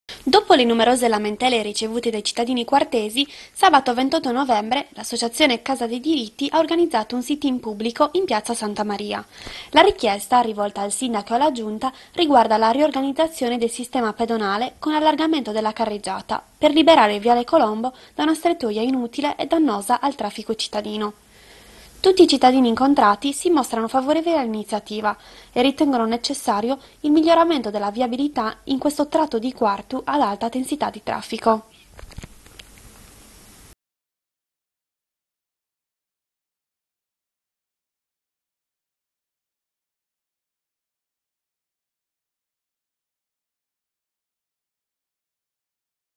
sit in per l'allargamento della carreggiata
sabato 28 novembre 2009 si è tenuto in piazza Santa Maria a Quartu un sit in per chiedere al sidaco e alla giunta l'allargamento della carreggiata in viale Colombo(angolo via Rssini) per migliorare la viabilità in questo tratto di strada ad alta intensità di traffico